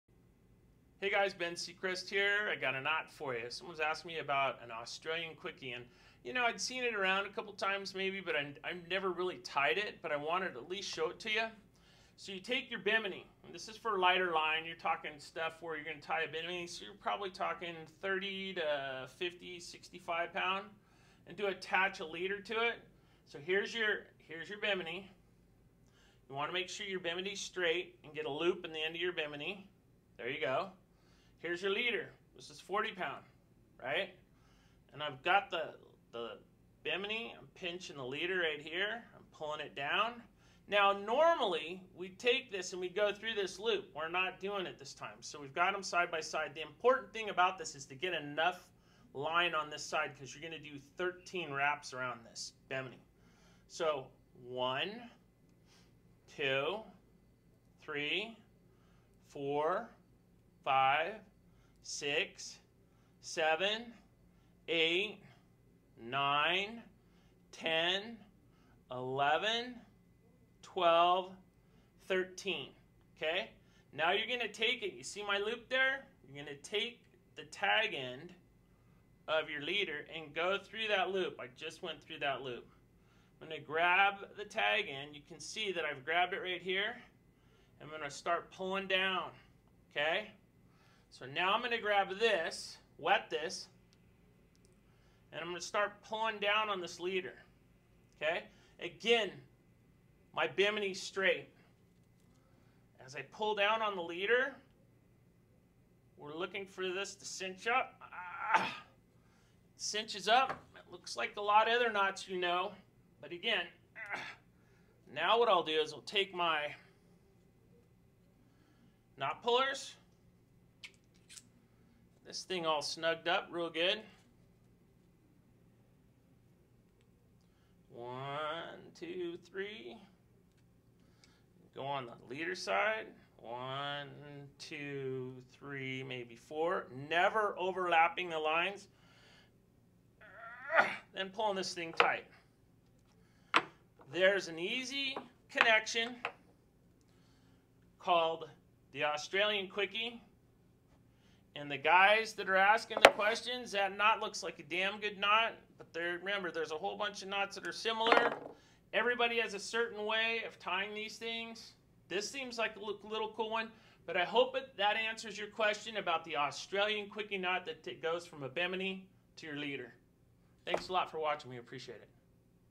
How to tie an Australian Quickie Knot
Ben Secrest breaks down how to tie the Australian Quickie knot.  Watch the video above for step by step on how to tie this easy knot - ACCURATE

ACCURATE FISHING
Want to reach us?!